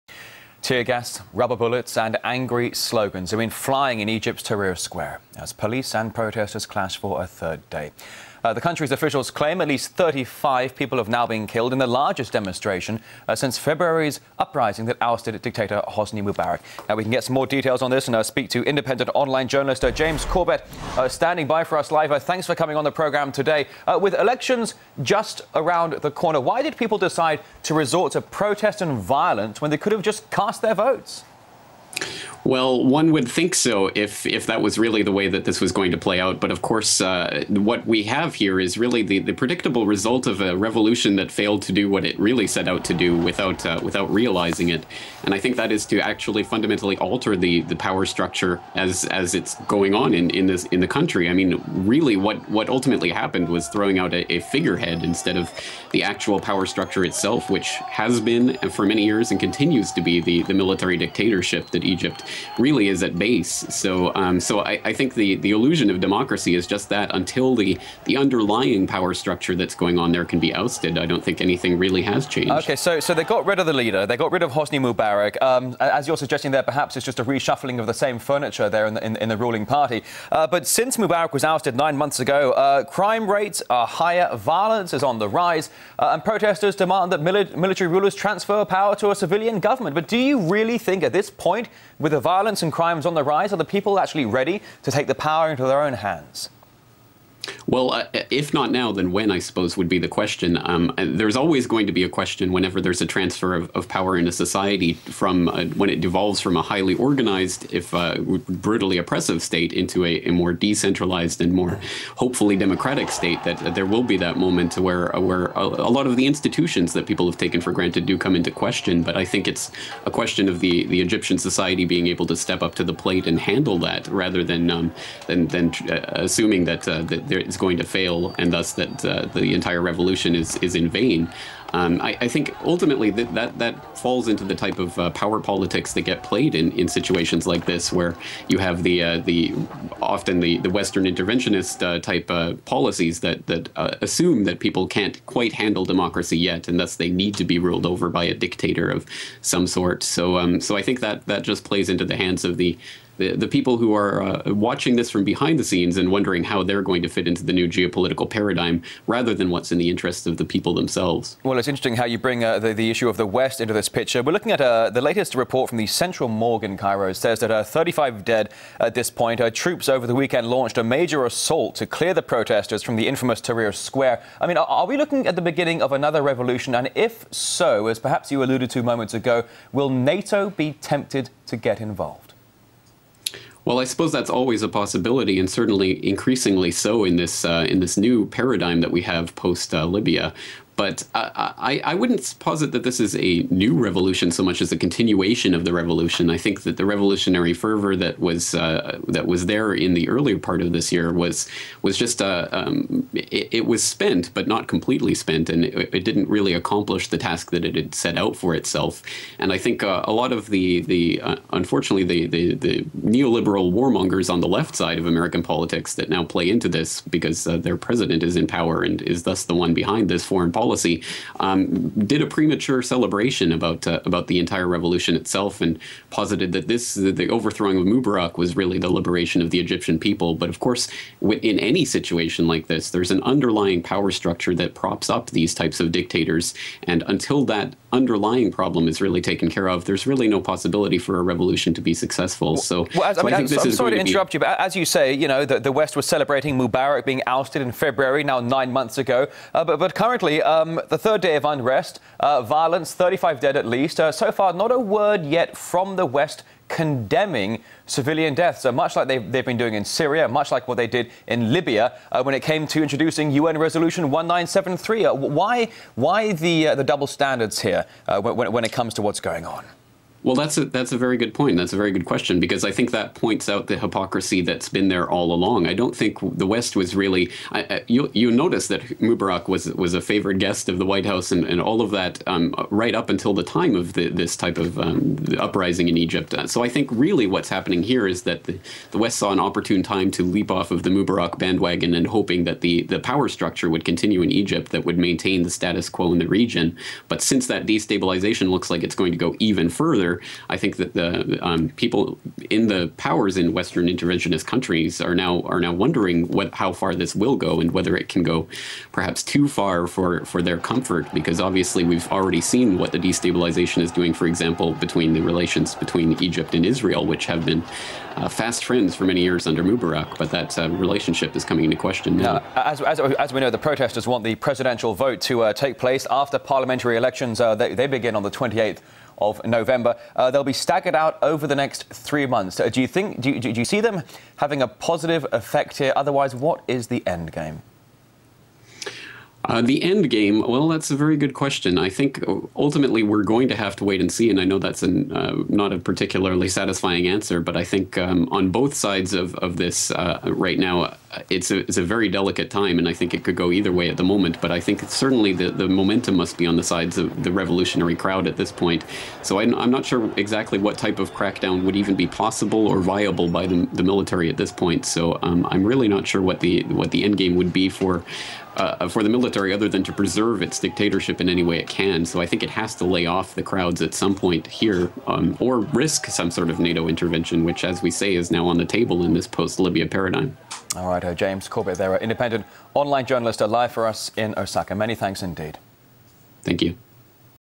Egypt junta options- Tahrir tear gas or NATO bombs
Tear gas, rubber bullets and angry slogans have been flying in Egypt's Tahrir Square as police and protesters clash for a third day. The country's officials claim at least 35 people have been killed in the largest demonstration since February's uprising that ousted dictator Hosni Mubarak. Independent online journalist James Corbett says spring revolt threw out a figurehead but didn't achieve structural changes in military power structure.